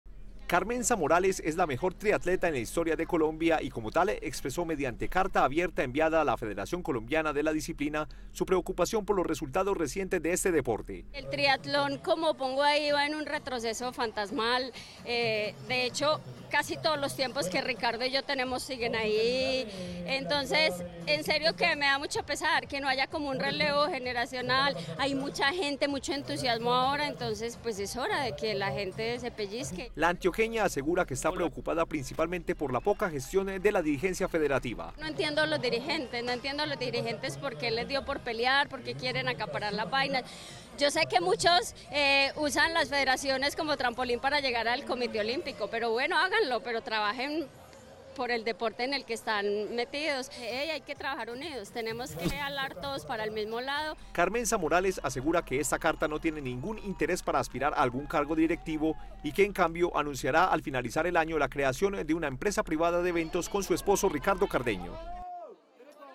La Triatleta Carmenza Morales envió un mensaje a la Federación [Noticias] - TeleMedellin
Carmenza Morales es la mejor triatleta en la historia de Colombia y como tal, expresó mediante carta abierta enviada a la Federación Colombiana de la disciplina su preocupación por los resultados recientes de este deporte.